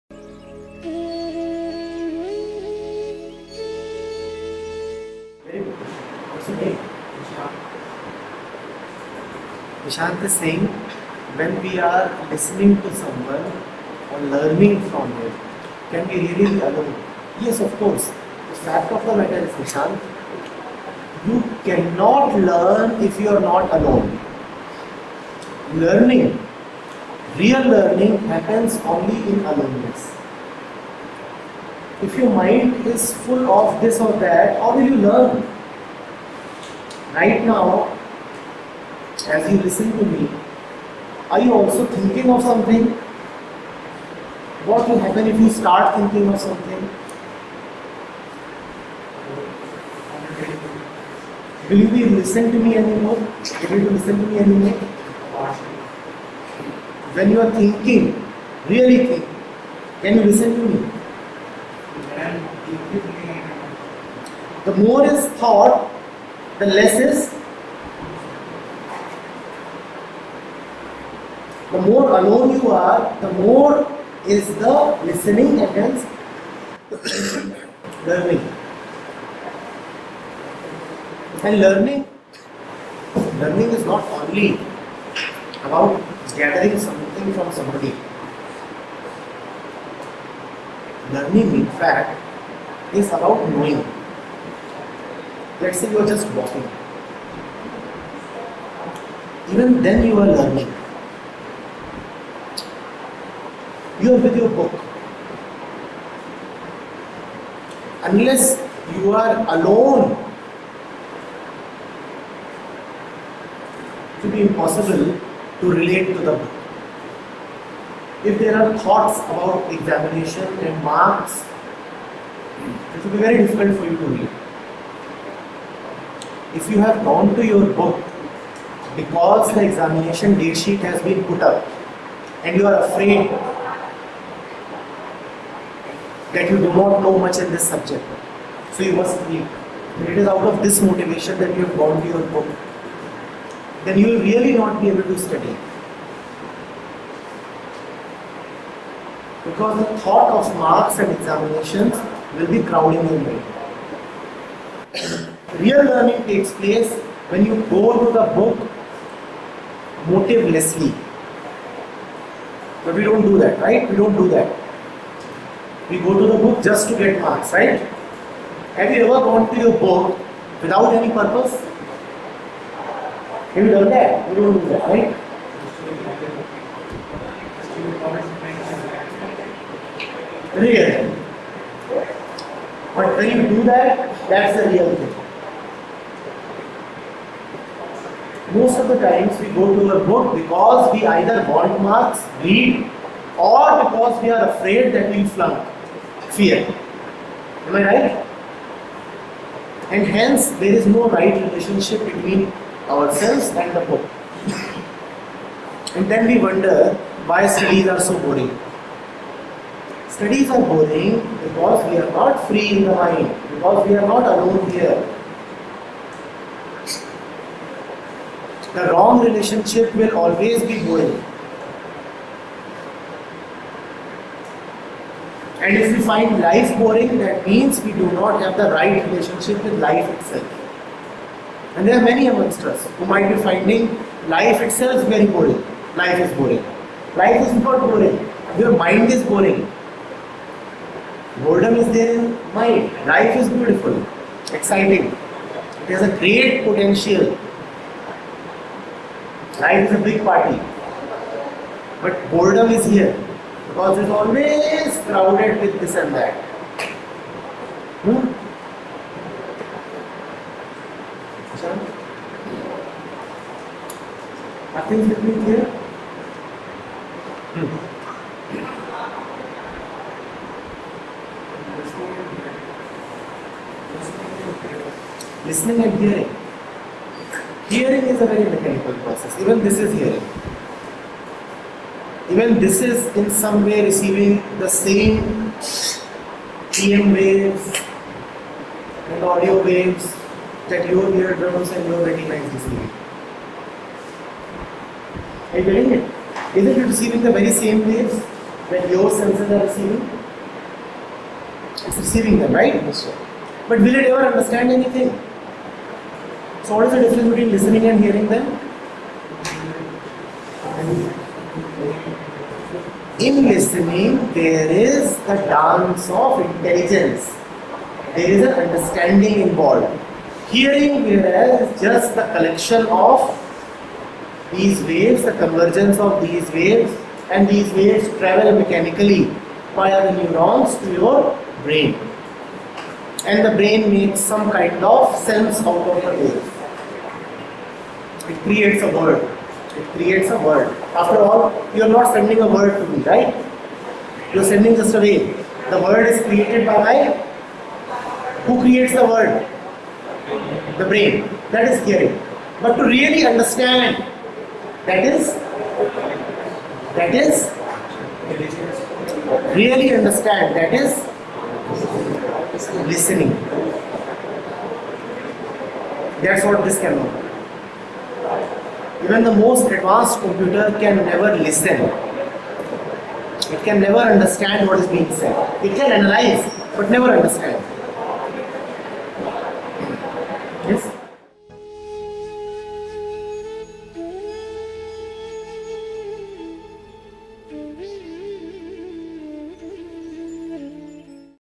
Listening happens in aloneness || Acharya Prashant, with youth (2013)
Video Information: Samvaad Session, 20.3.13, Gurugram, Haryana, India

Context:
~ How real Learning Happens?
~ Is thinking and listening go together?